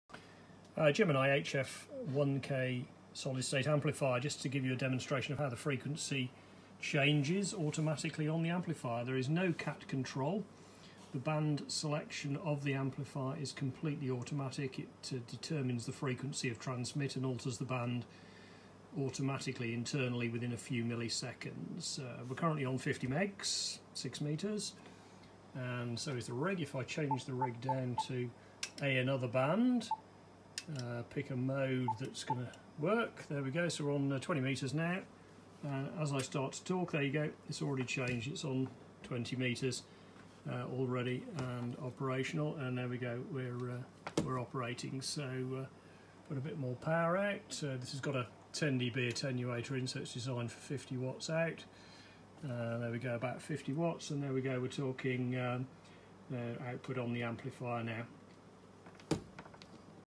Gemini HF-1K amplifier band selection
Demo of fully automatic band selection on the Gemini HF-1K amplifier.